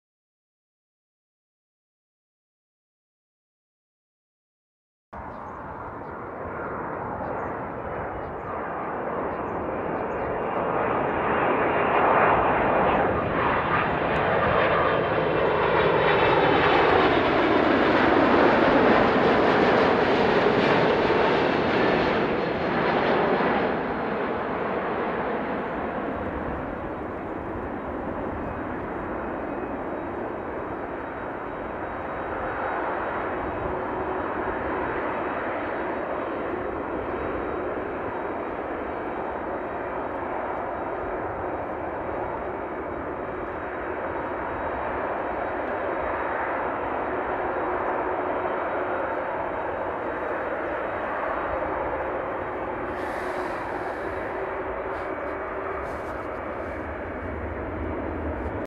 ANA Cargo B-767 着陸@成田空港(NRT)
2009年3月 成田空港 ANA Cargo B-767 着陸
RW34Lエンドで撮影